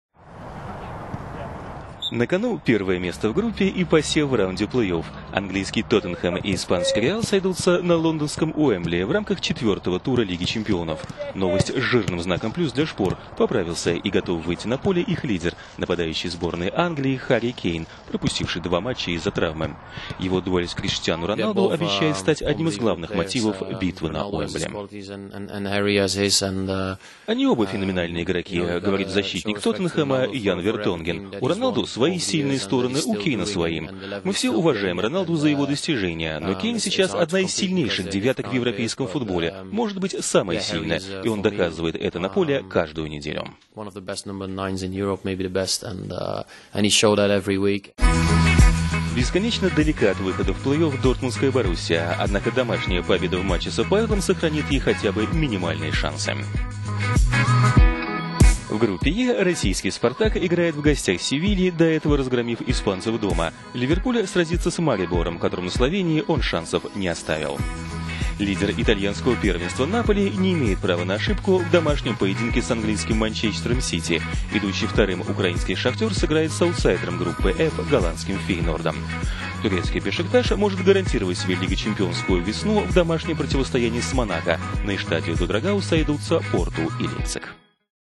Лига чемпионов: битва на "Уэмбли"
На кону первое место в группе и "посев" в раунде плей-офф. Английский "Тоттенхэм" и испанский "Реал" сойдутся на лондонском "Уэмбли" в рамках четвертого тура Лиги чемпионов. Новость с жирным знаком "плюс" для "Шпор" - поправился и готов выйти на поле их лидер, нападающий сборной Англии Харри Кейн, пропустивший два матча из-за травмы. Его дуэль с Криштиану Роналду обещает стать одним из главных мотивов битвы на "Уэмбли".

"Они оба феноменальные игроки, - говорит защитник "Тоттенхэма" Ян Вертонге…